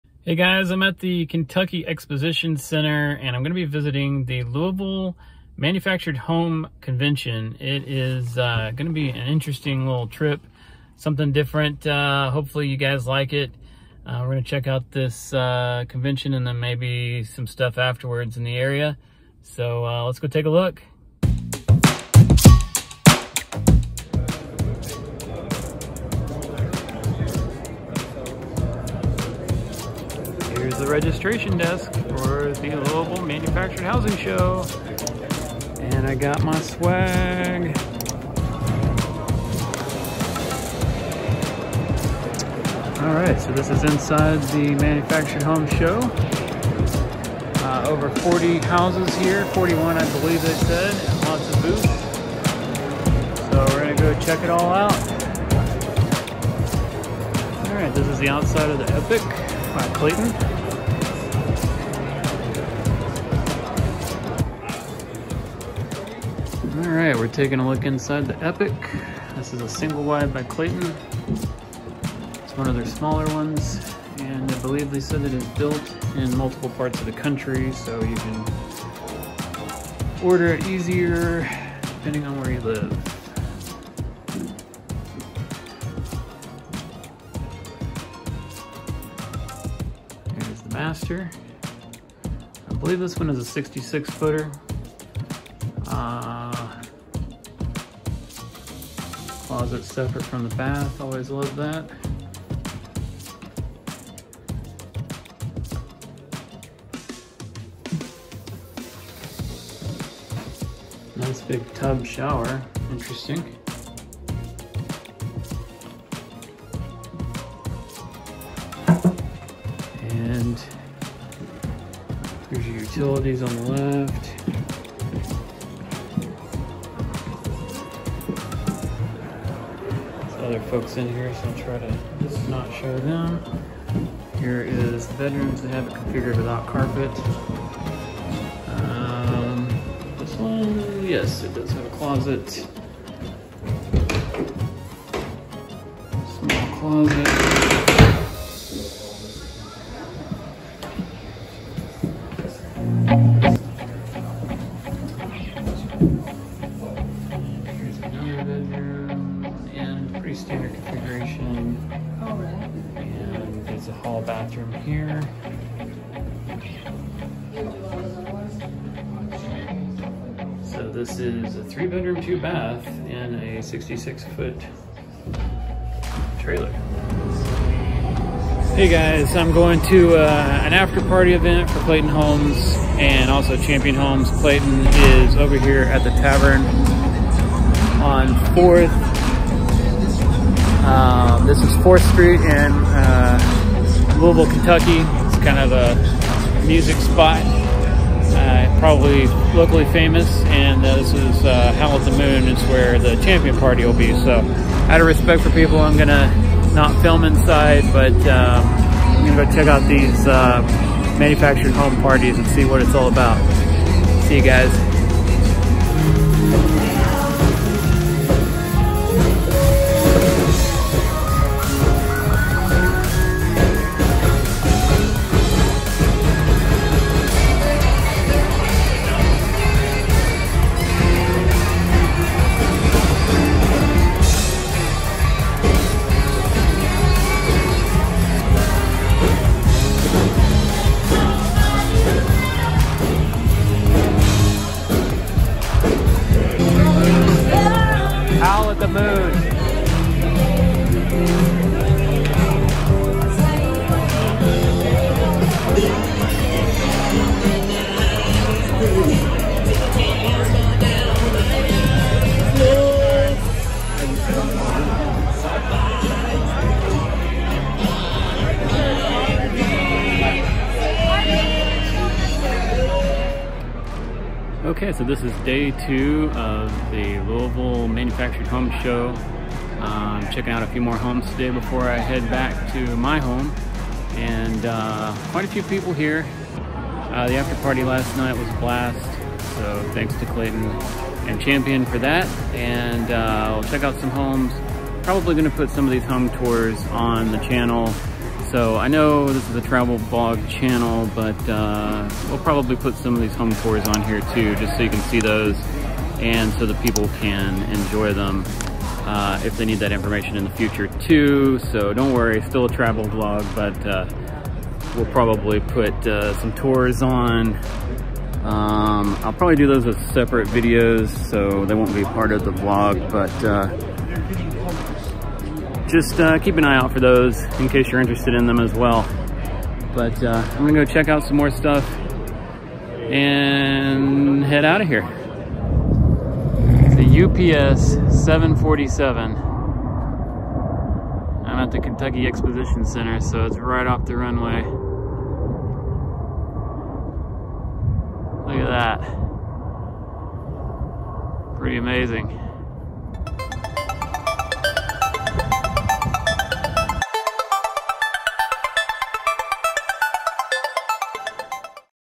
Louisville Manufactured Home Show 2024 VLOG
Parker went to the Louisville Kentucky Manufactured Home Show to do some fact finding and check out some new homes. Come along on this adventure crossing through 4 states to spend 2 days at the show.